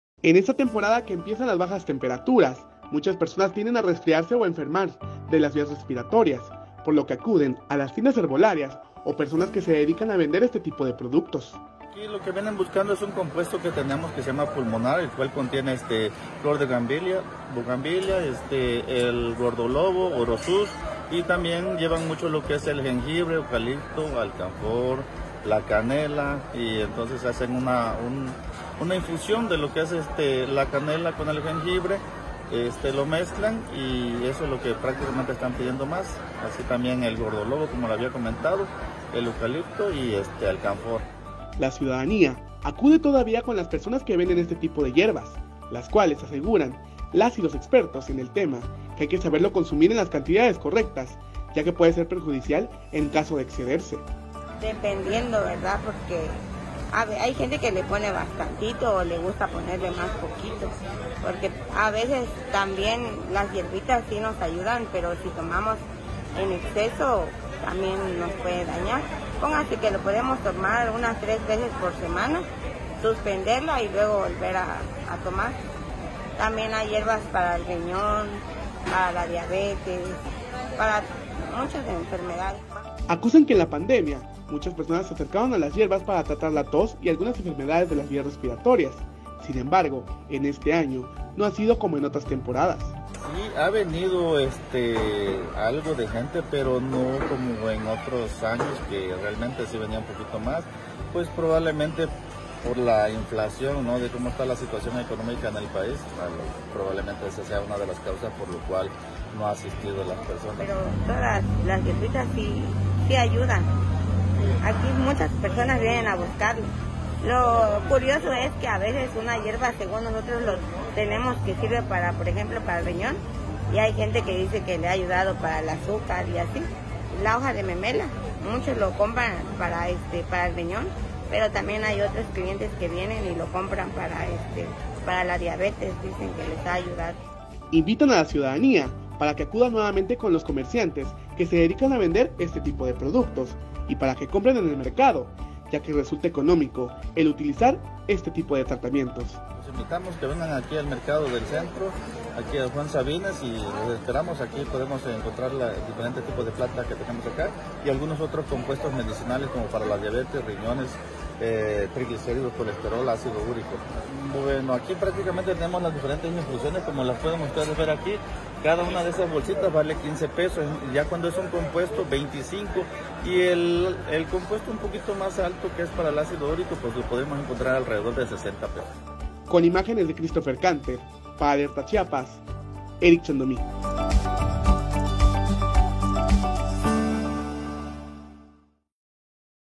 Para este friecito un rico tecito
Es tiempo de catarros y resfriados, para el alivio la herbolaria es una opción, pero es importanre usar las porciones correctar, opina un experto.